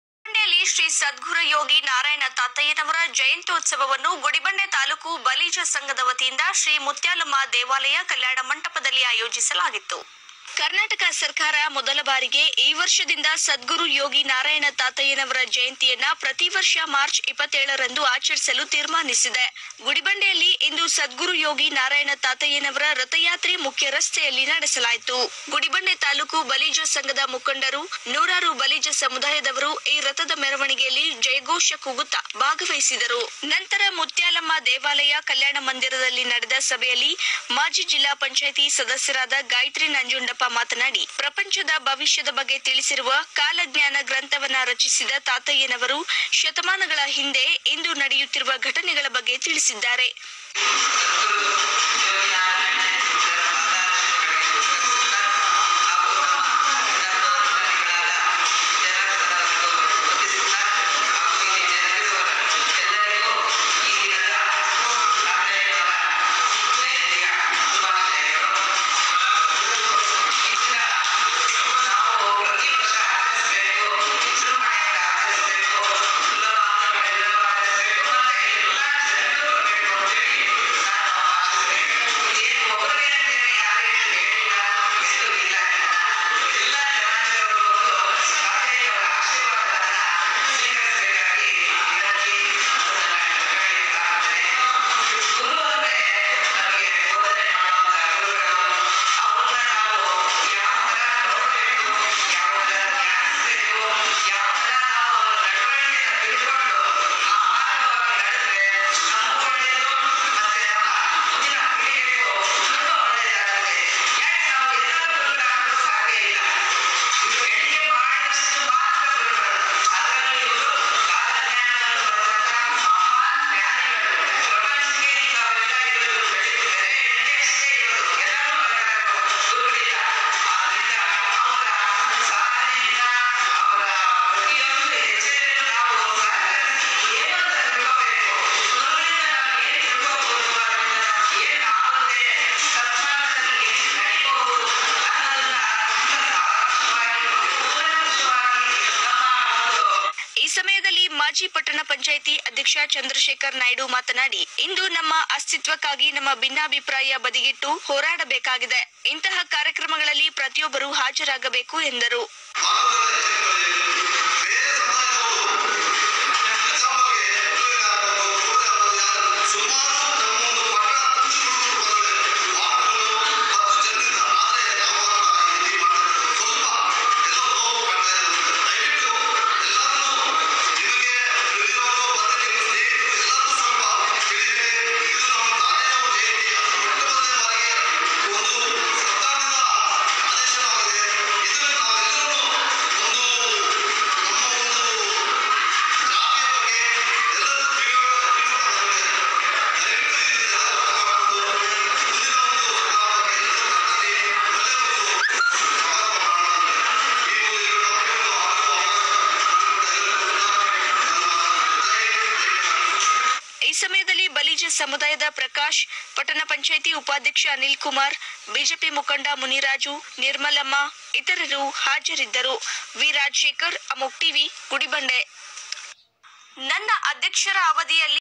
ಗುಡಿಬಂಡೆ ತಾಲ್ಲೂಕು ಬಲಿಜ ಸಂಘದ ವತಿಯಿಂದ ಸದ್ಗುರು ಯೋಗಿ ನಾರೇಯಣ ತಾತಯ್ಯನವರ ಜಯಂತ್ಯುತ್ಸವ.
ವಿ.ರಾಜಶೇಖರ್. ಅಮೋಘ ಟಿವಿ. ಗುಡಿಬಂಡೆ